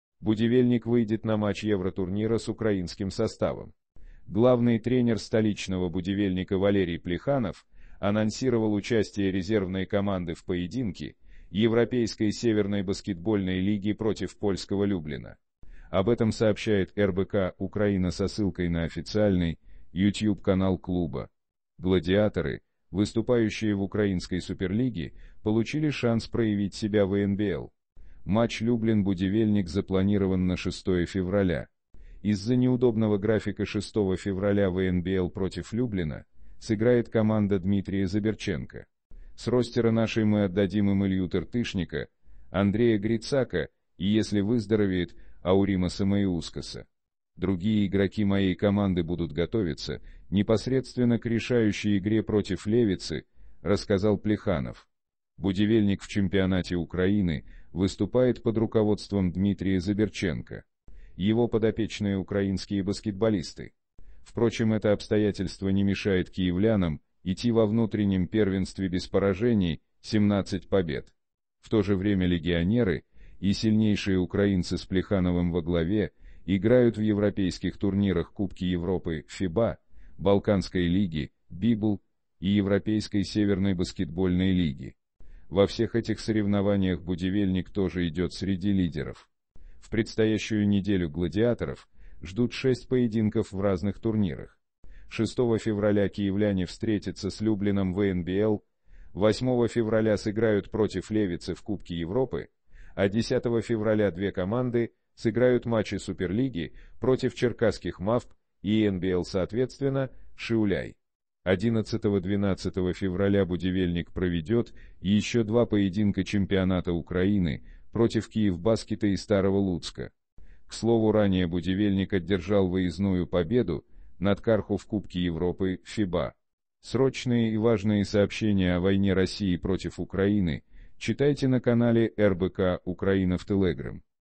"Будивельник" выйдет на матч евротурнира с "украинским" составом.
Баскетбол Главный тренер столичного "Будивельника" Валерий Плеханов анонсировал участие резервной команды в поединке Европейской Северной баскетбольной лиги против польского "Люблина". Об этом сообщает РБК-Украина со ссылкой на официальный YouTube-канал клуба. "Гладиаторы", выступающие в украинской Суперлиге, получили шанс проявить себя в ENBL. Матч "Люблин" – "Будивельник" запланирован на 6 февраля. "Из-за неудобного графика 6 февраля в ENBL против "Люблина" сыграет команда Дмитрия Забирченко. С ростера наш